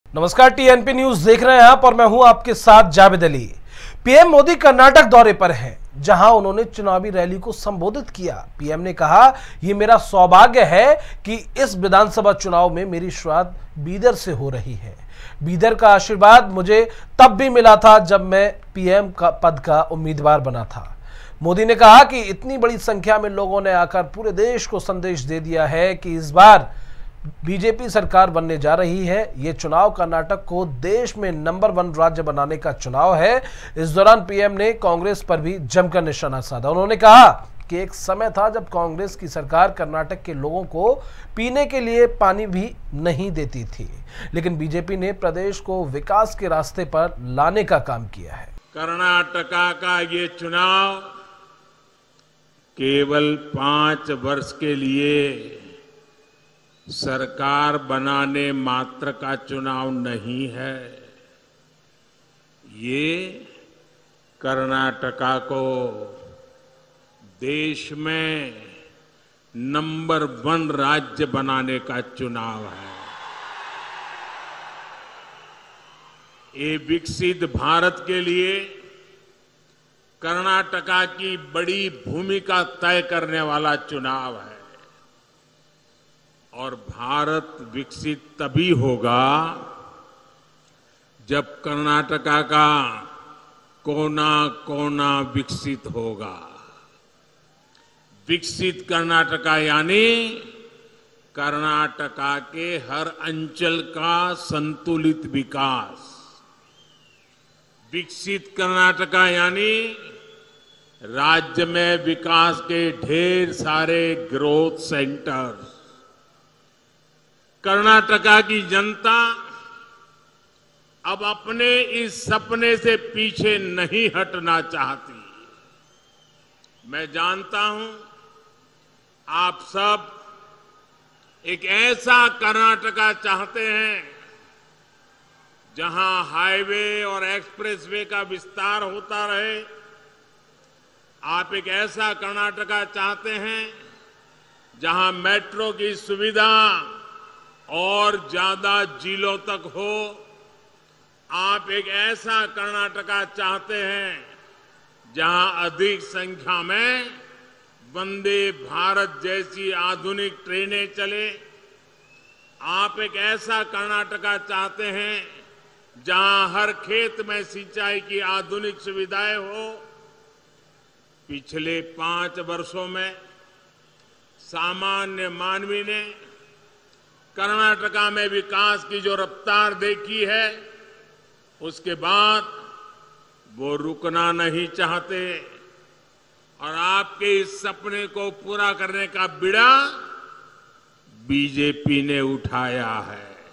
PM Modi Rally in Karnataka: कर्नाटक में पीएम मोदी की हुंकारइस बार भी बनेगी बीजेपी सरकार | TNP News
प्रधानमंत्री नरेंद्र मोदी ने शनिवार को कर्नाटक में अपनी पहली चुनावी रैली को संबोधित किया। उनकी पहली रैली बीदर के हुमनाबाद में हुई है, जहां उन्हें सुनने के लिए बड़ी संख्या में लोग इकट्ठा हुए।

यहां पीएम ने कहा, "ये मेरा सौभाग्य है कि इस विधानसभा चुनाव में मेरी शुरुआत बीदर से हो रही है। बीदर का आशीर्वाद मुझे तब भी मिला था जब मैं पीएम पद का उम्मीदवार बना था। आज इतनी बड़ी संख्या में यहां आ कर आपने पूरे देश को संदेश दे दिया है कि- इस बार, भाजपा सरकार। यह चुनाव कर्नाटक को देश में नंबर-1 राज्य बनाने का चुनाव है।"